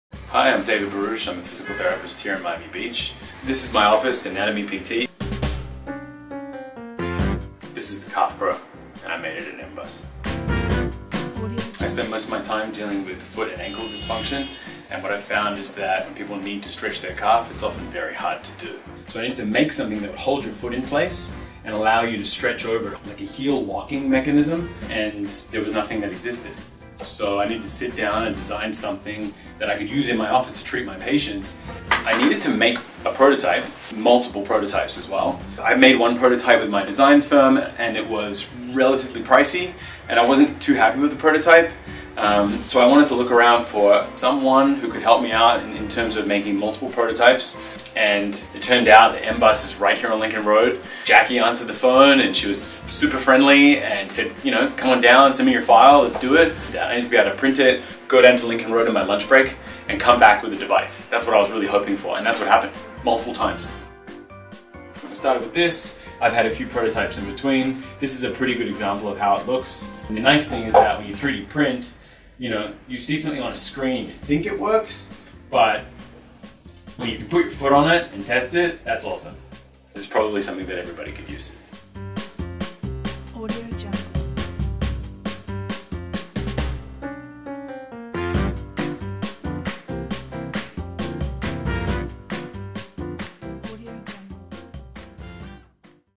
3D Printing and Rapid Prototyping: David Barouche and the Calf Pro
Physical Therapist David Barouche needed a way to accelerate  product design for his calf-stretching device, The Calf Pro. Find out how we, at FIU Miami Beach Urban Studios, helped him bring his vision to life in one of the largest 3D printing facilities in the nation - our very own Innovation Lab

Happened at FIU Miami Beach Urban Studios